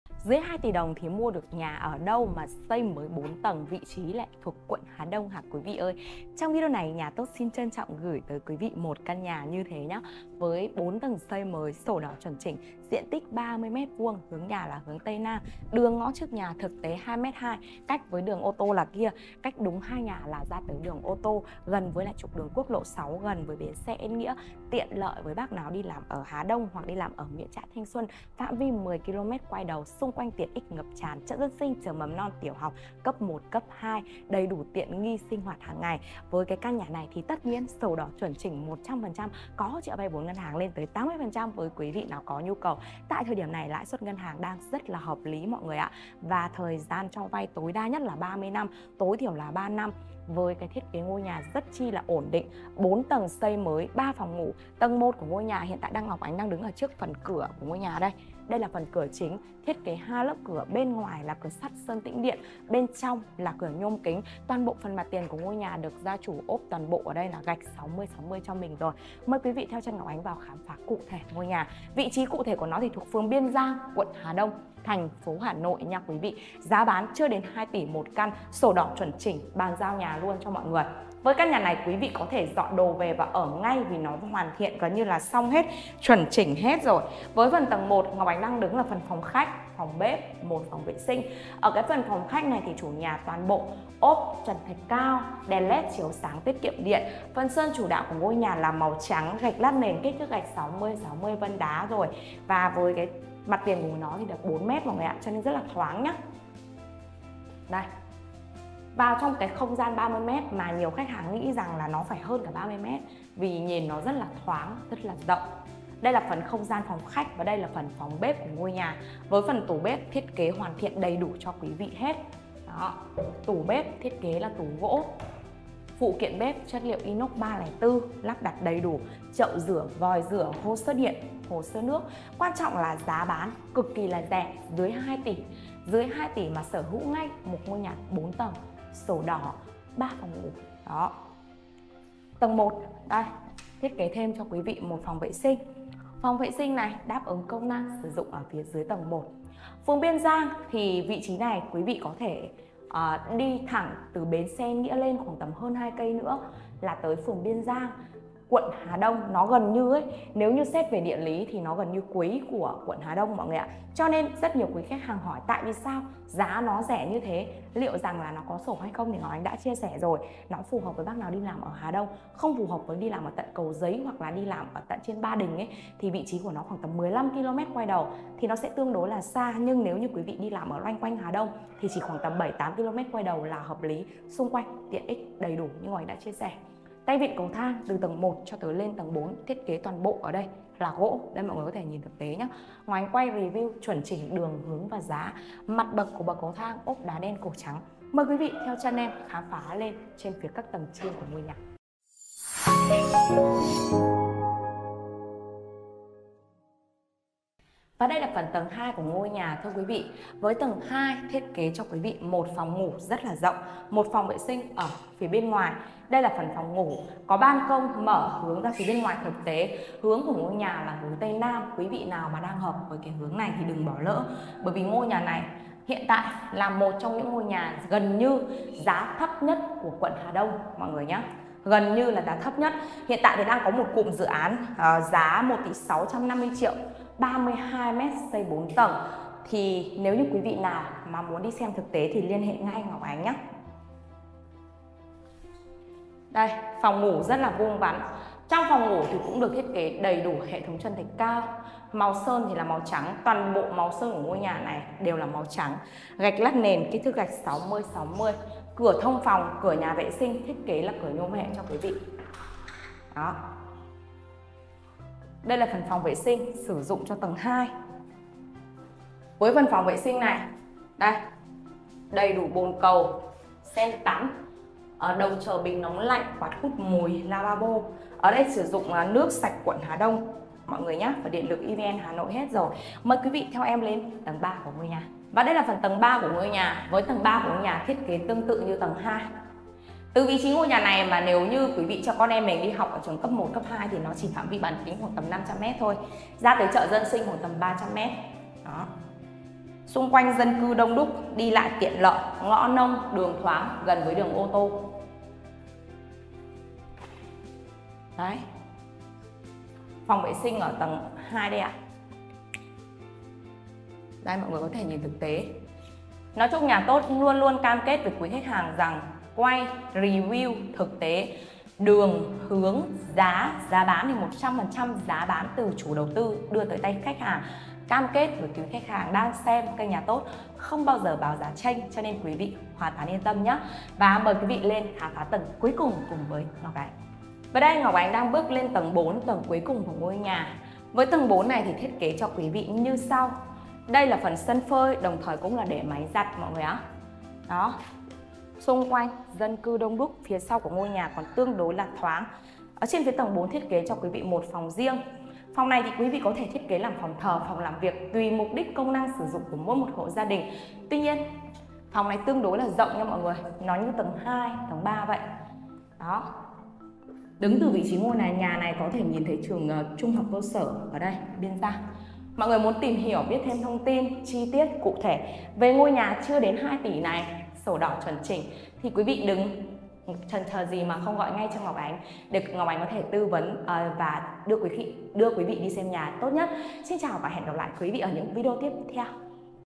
Bán nhà 4 tầng quận Hà Đông gần trường học, chợ, bến xe, ql6 | nhà tốt
- Địa chỉ: Biên Giang, Hà Đông, Hà Nội
- Diện tích: 30m2
- Hướng nhà: Tây Nam
- Thiết kế: 4 tầng, 3PN, 3wc
- Giá bán: 1,79 tỷ